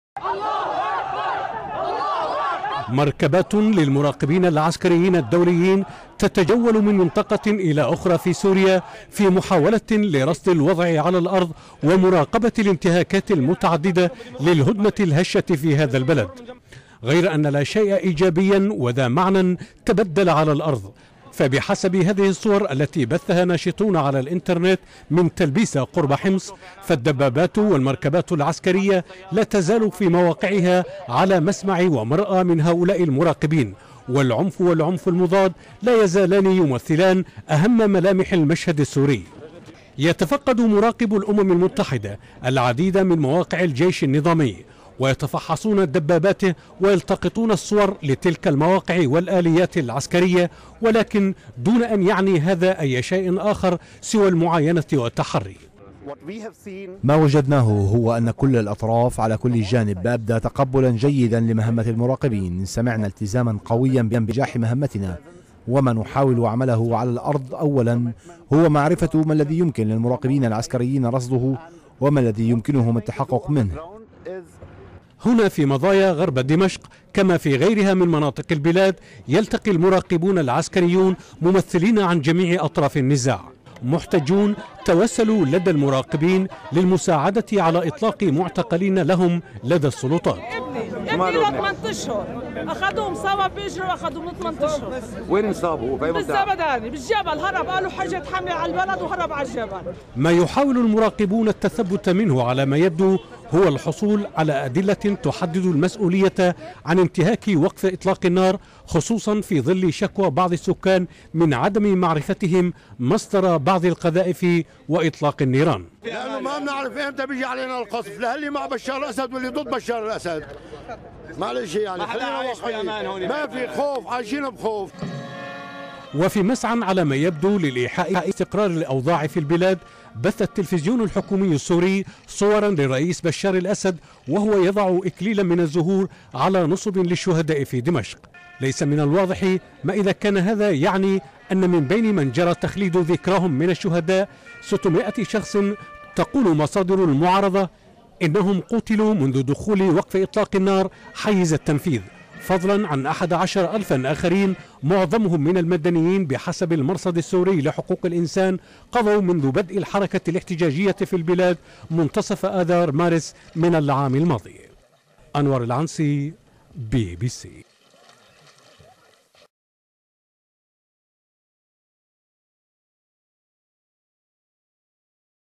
المعارضة السورية تدعو إلى مقاطعة الانتخابات التشريعية
دعت المعارضة السورية الى مقاطعة الانتخابات التشريعية المقرر اجراؤها يوم غد وذلك في وقت استمرت فيه الاحتجاجات والتظاهرات المطالبة باسقاط النظام.